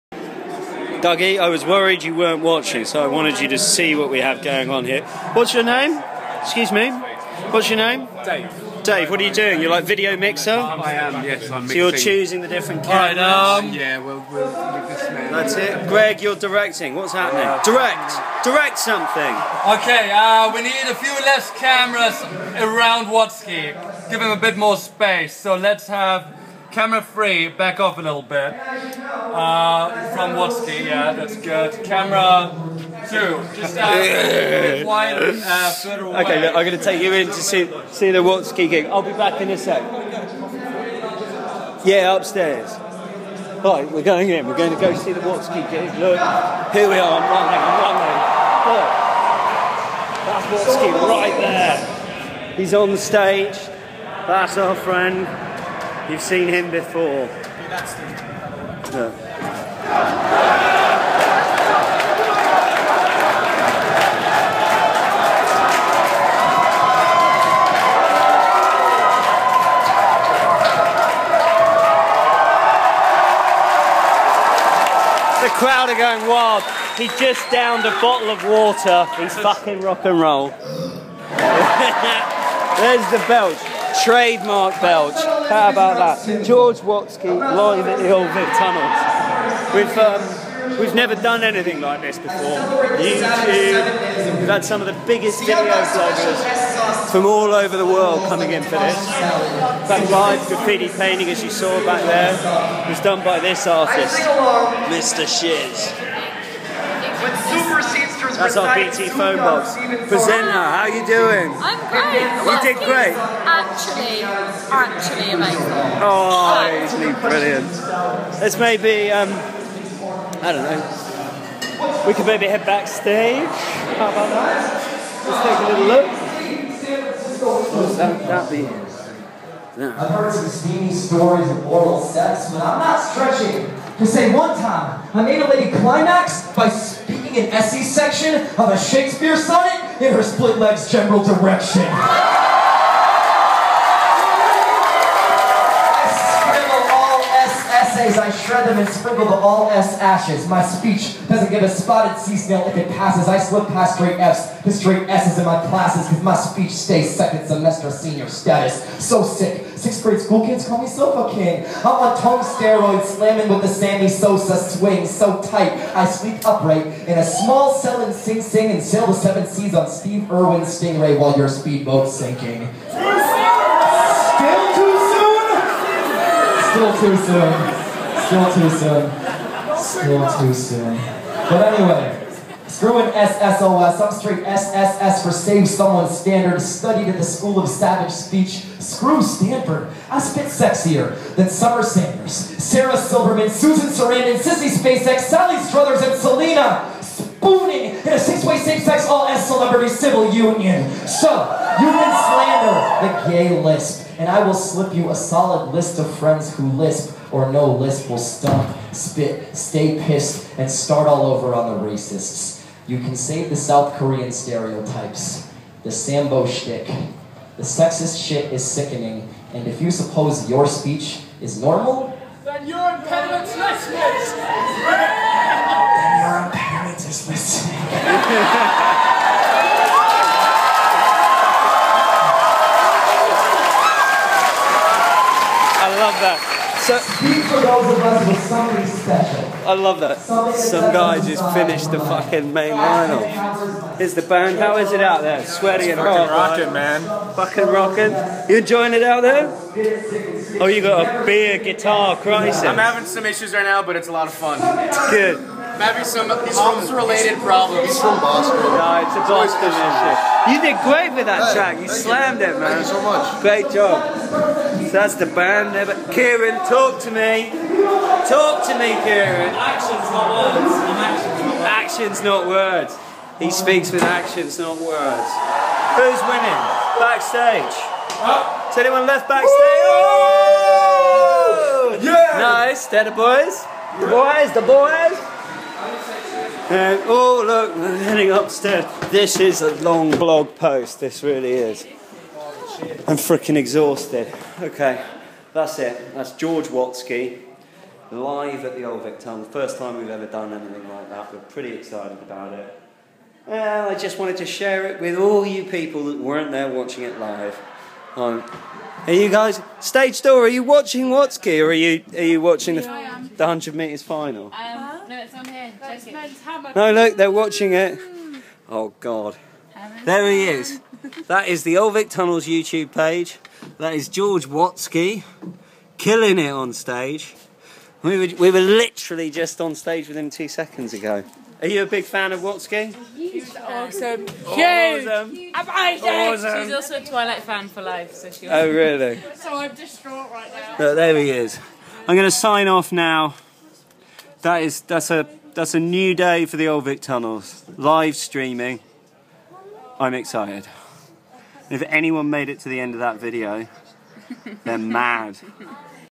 Watsky Live at The Old Vic Tunnels
A trip back stage at the Watsky gig at The Old Vic Tunnels.